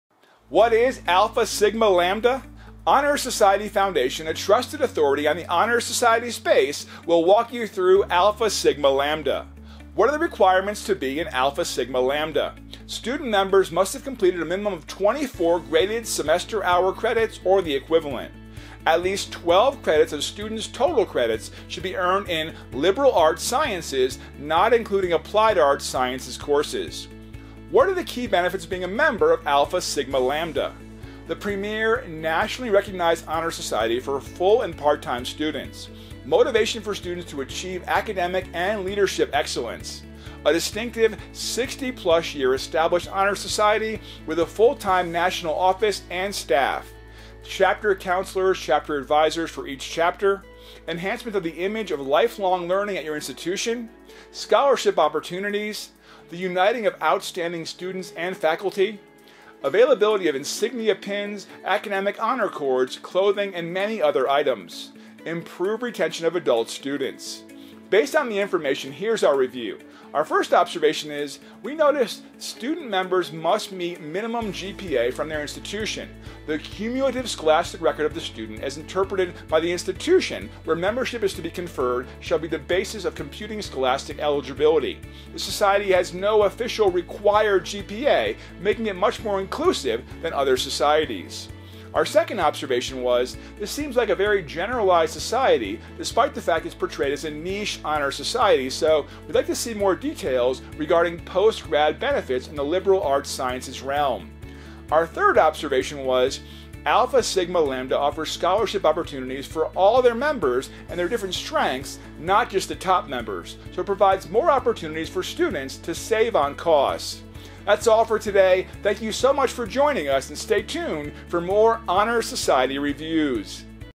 Alpha Sigma Lambda Honor Society Review & Requirements
What is Alpha Sigma Lambda? Honor Society Foundation, a trusted authority on the honor society space, will walk you through Alpha Sigma Lambda.

What are the requirements to be in Alpha Sigma Lambda?

-Student members must have completed a minimum of twenty-four (24) graded semester hour credits or the equivalent
-At least twelve credits of student’s total credits should be earned in courses in Liberal Arts/Sciences, not including applied Arts/Sciences courses.

What are the key benefits of being a member of Alpha Sigma Lambda?

-The premier nationally recognized honor society for full- and part-time students.
-Motivation for students to achieve academic and leadership excellence.
-A distinctive, 60+ year established honor society with a full-time national office and staff.
-Enhancement of the image of lifelong learning at your institution.
-Scholarship opportunities.